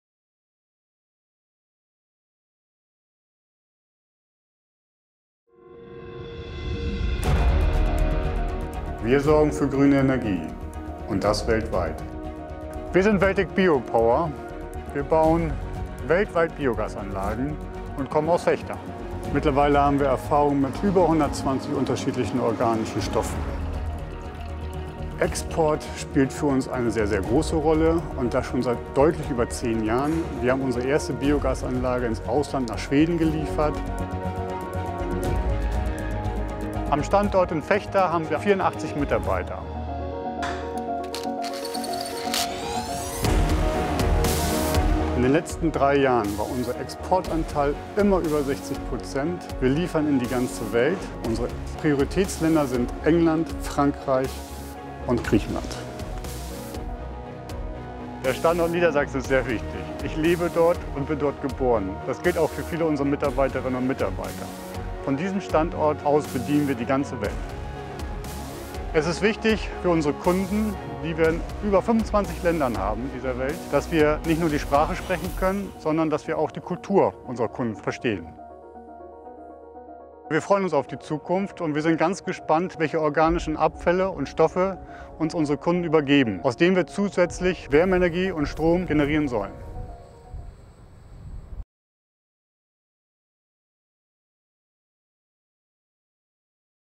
Außenwirtschaftspreis 2017 - die Nominierten: Weltec Biopower
Weltec Biopower wurde 2017 für den niedersächsischen Außenwirtschaftspreis nominiert. Mehr über den Außenwirtschaftspreis und den Wirtschaftsstandort Niedersachsen finden Sie